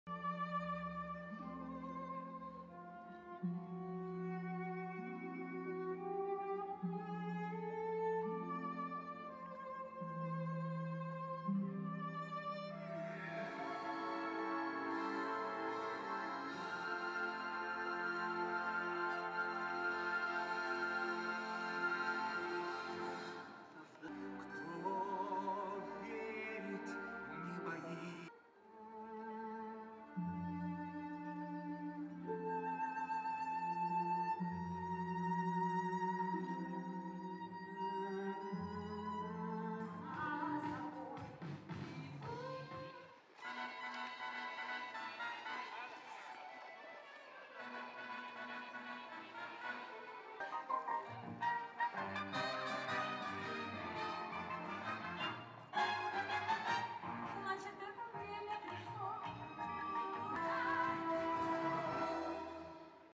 Olympic dancer opens iconic Red Square ice rink
Russia’s iconic Red Square ice skating rink, a favorite among Muscovites, opened for the season with a colorful show featuring 2006 Olympic ice dancing champion Tatyana Navka.